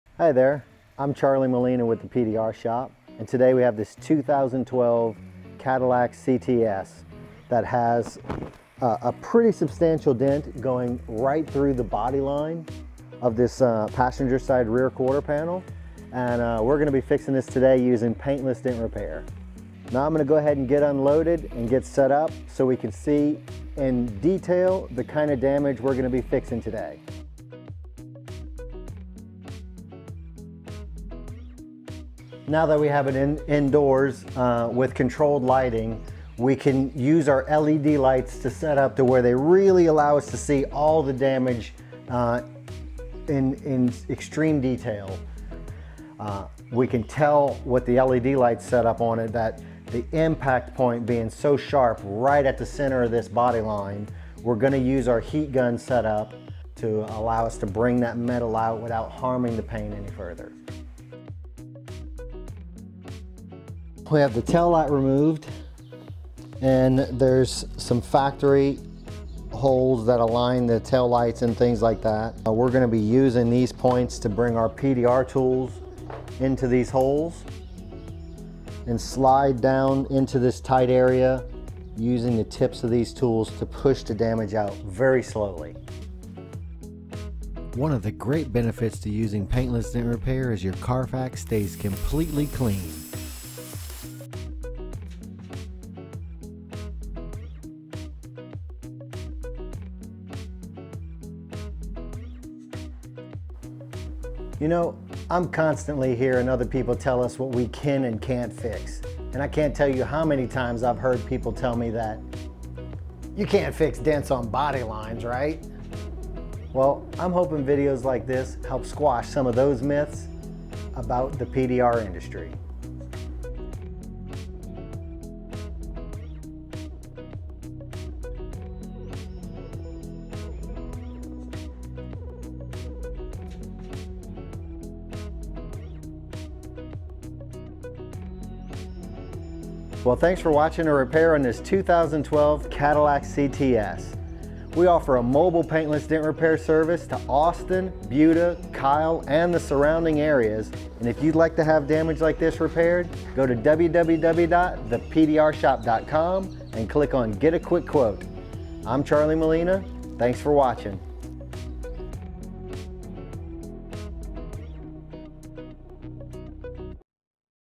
2012 Cadilac CTS Body Line Dent MobilePaintless Dent Repair Kyle TX, Buda TX, Austin TX
The PDR Shop offers mobile Paintless Dent Repiar to Kyle Tx, Buda TX, Austin TX, and the surrounding areas.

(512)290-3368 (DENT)